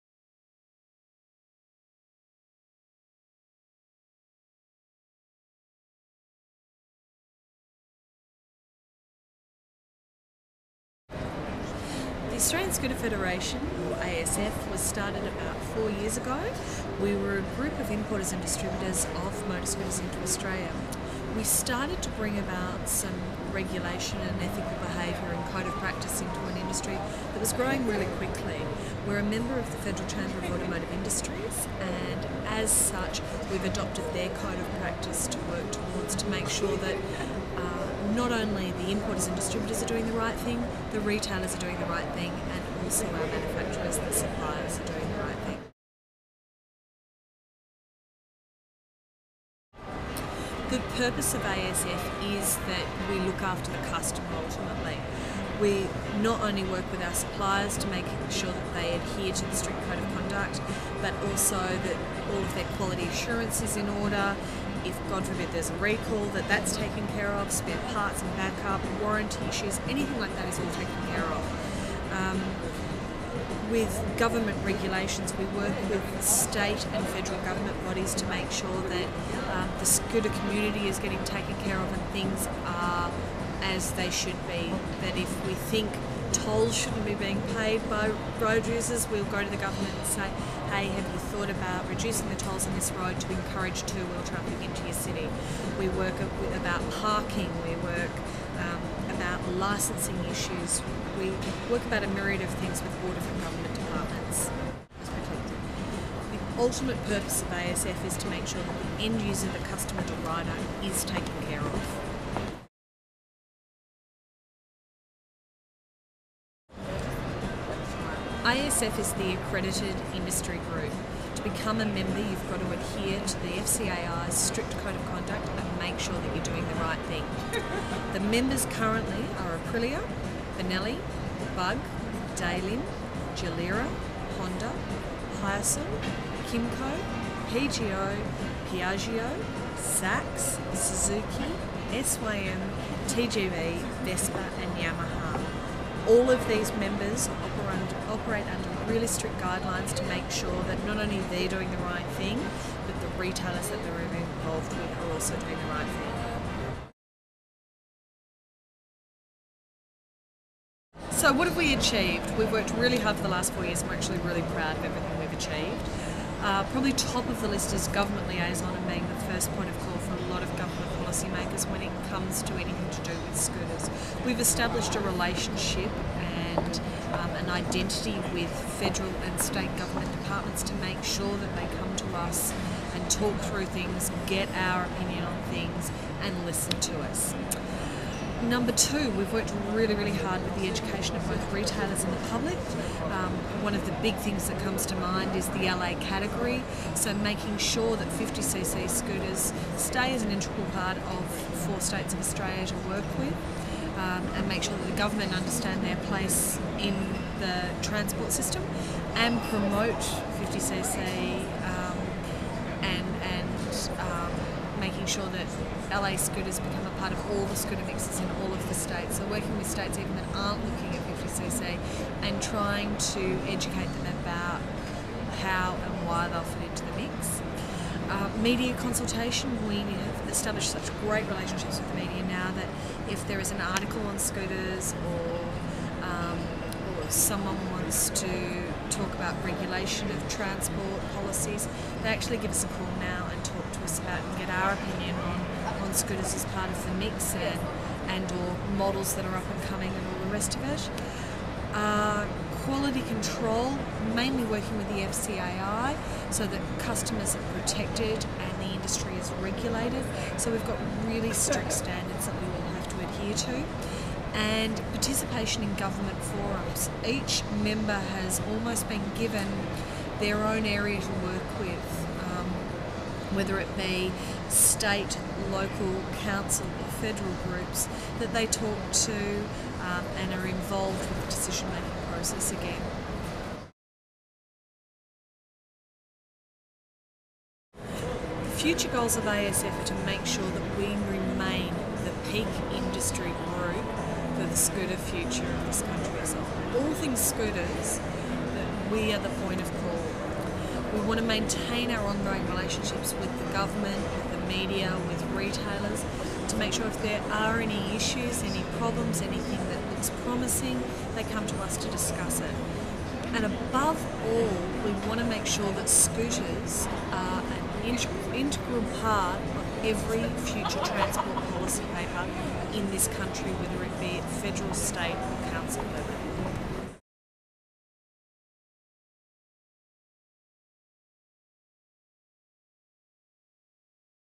Hollie Black on the Australian Scooter Federation
Hollie Black is the Chairperson of the Australian Scooter Federation. Here she takes us through just who is the ASF, some of the functions they perform and where to it is from here. Visit Scootersales.com.au, Australia's number one website news, reviews and classified scooter website.